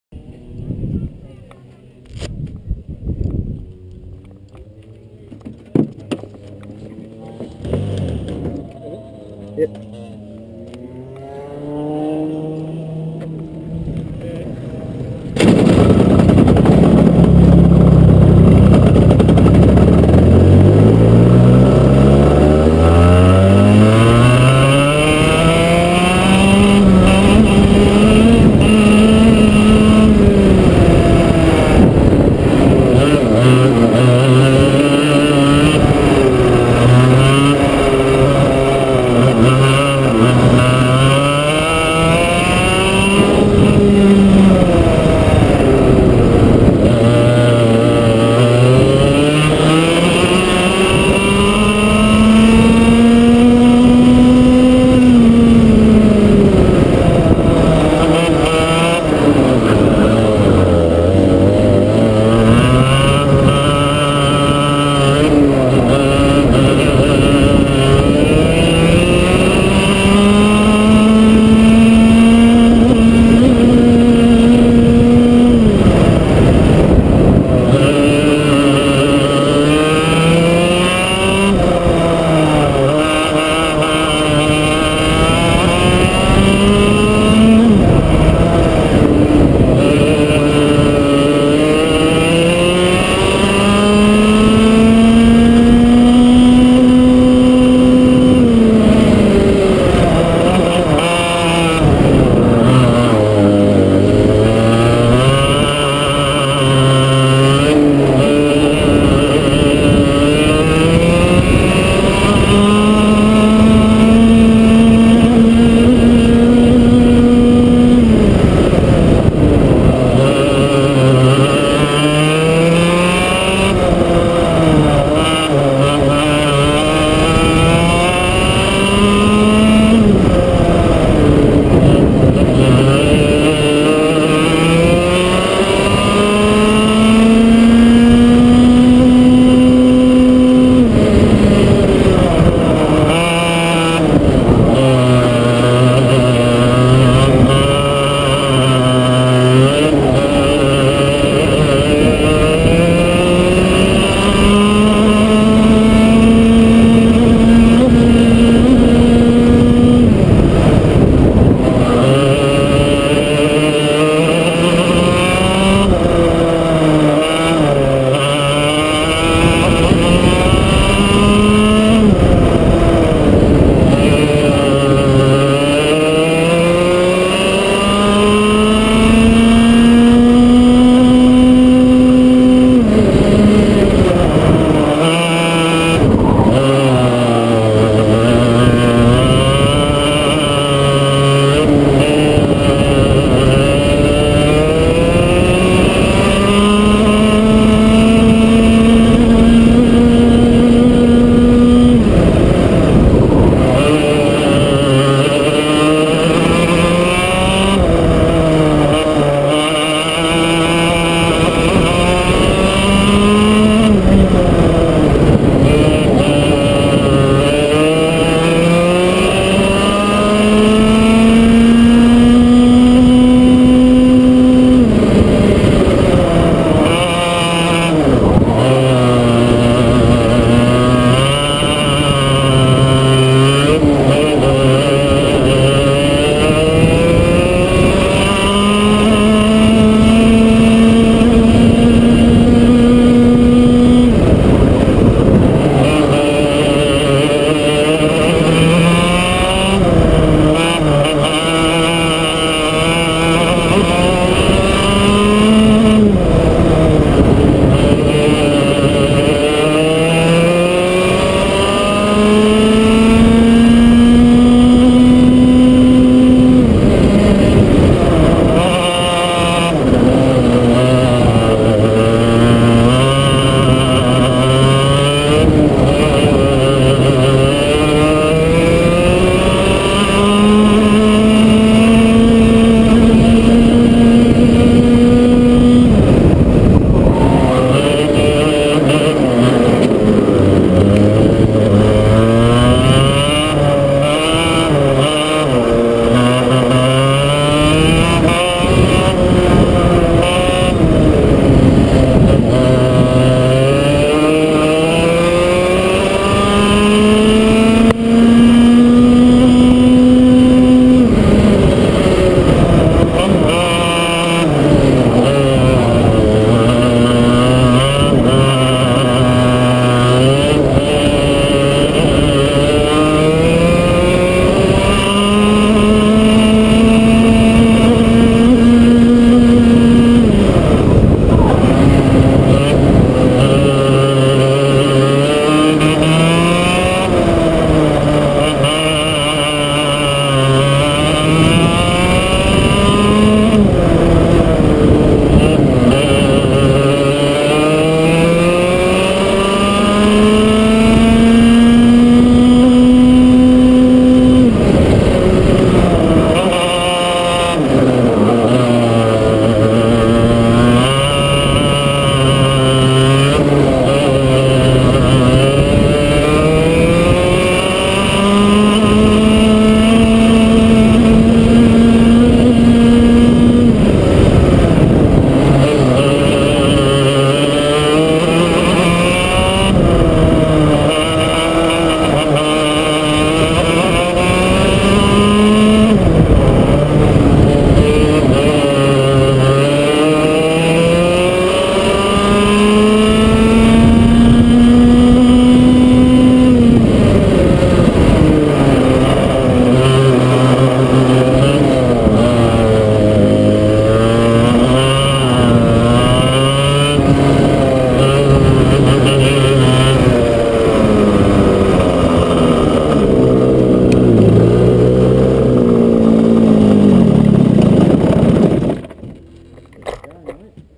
Stony Creek Go Karts GoPro HD HERO 2 - 720p - 26-Jan-12 Arrow YAMAHA 100S
Stony Creek Go Karts 26-Jan-12 testing session
Victoria, Australia.
Arrow XE-28 with a yamaha 100s
GoPro HD HERO 2 - full HD - 720p
41.68 fastest time for the day